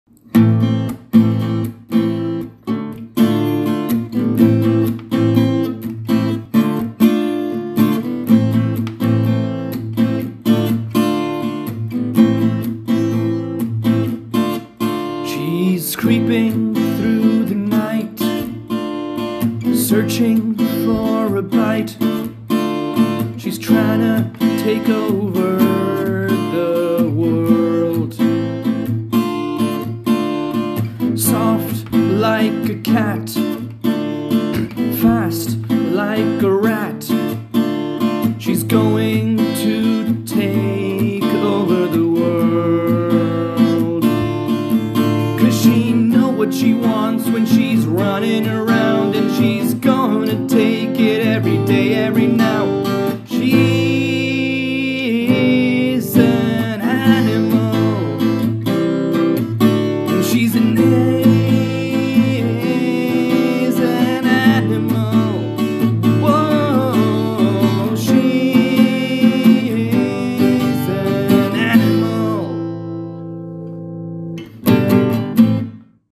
Improvising Songs for Girls on Dating Apps: Animal
🎵 Wait for it! 🎶 Shriyani on Hinge challenged me to improvise a song with the suggestion "Animal," leading to one of the most iconic rhymes in musical history...
Also had to fight the urge to just sing Maroon 5 😅